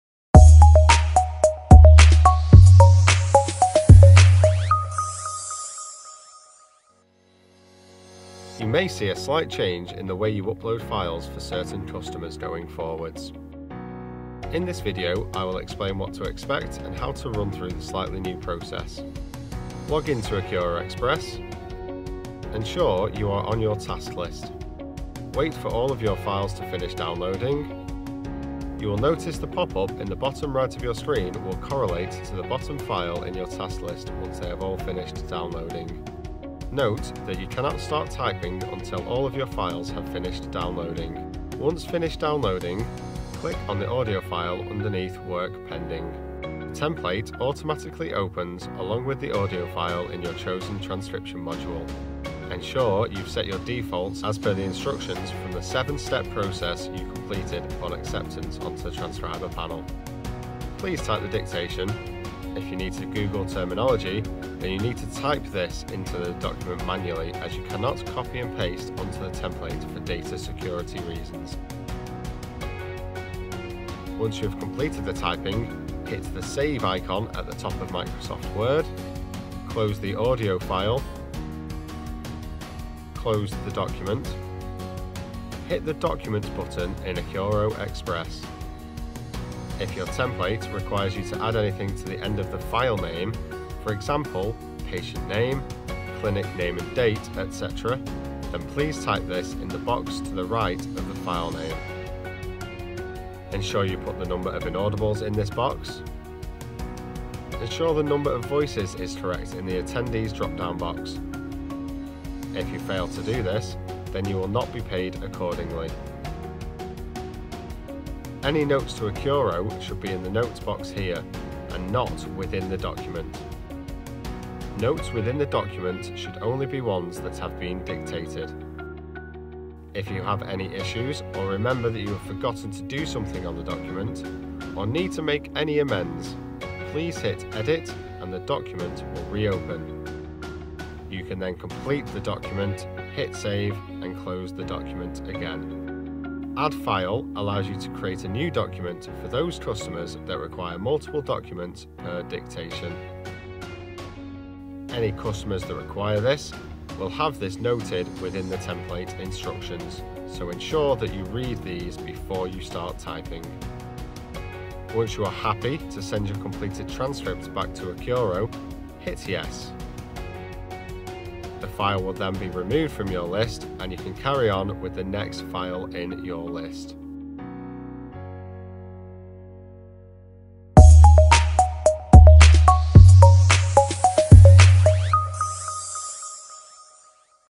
My File Says Documents | Transcribers | How to complete a file | Transcription
Step by step guide on how to upload files to Accuro when your file says Documents in Accuro Express.

If you haven't already signed up with Accuro, please send your application